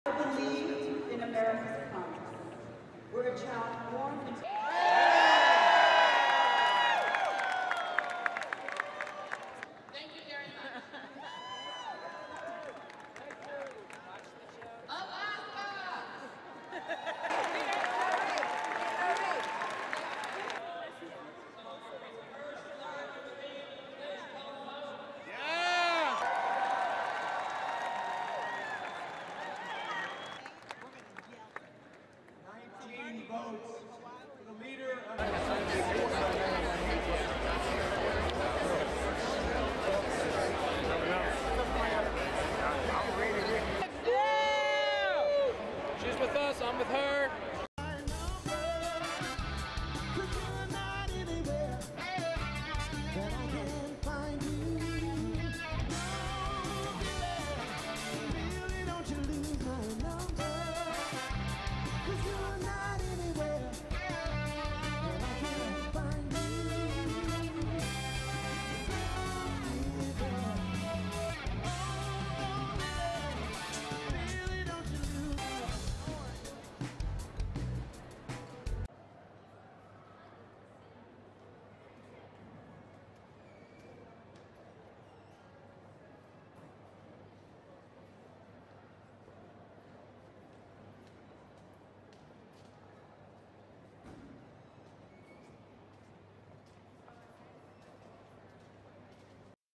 Democrats tip their hats to their nominee
Delegates at the Democratic National Convention on Tuesday (July 27) participated in the long-standing tradition of sporting eye-catching novelty hats on the convention floor.

As Hillary Clinton became the first woman presidential nominee of a major American party, her supporters decked their hats out in Hillary pins and ribbons, and novelty toys and knick-knacks.